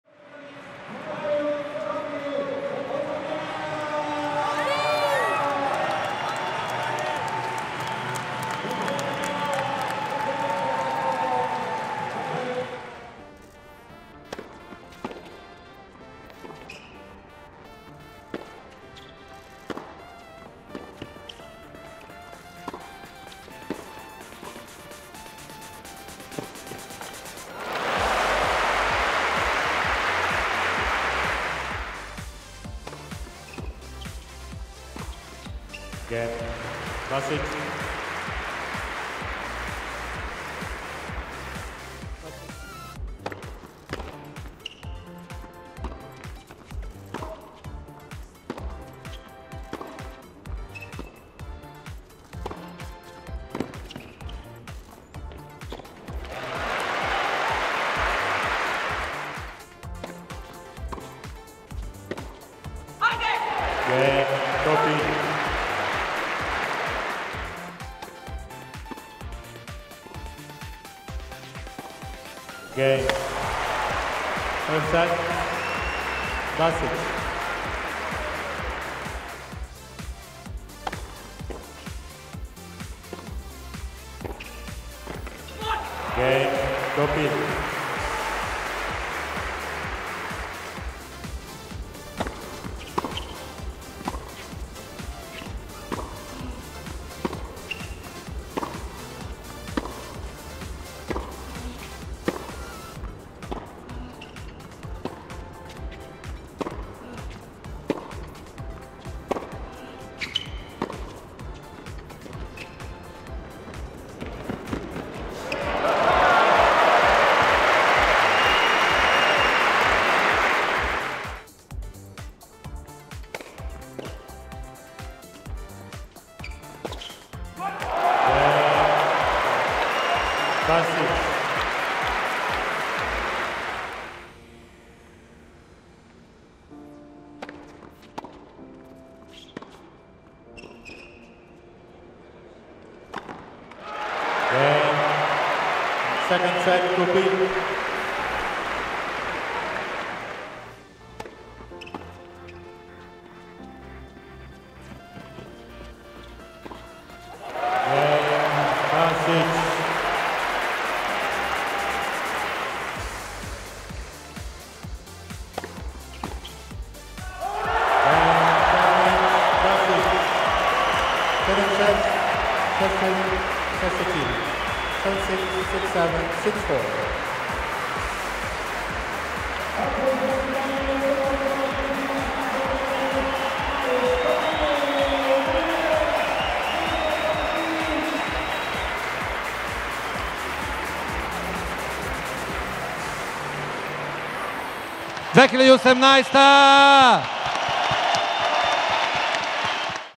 Highlights: Basic Wins First ATP Title At Sofia 2018
Watch highlights of the 2018 Diema Xtra Sofia Open final, featuring Mirza Basic and Marius Copil. Photo: Lap.bg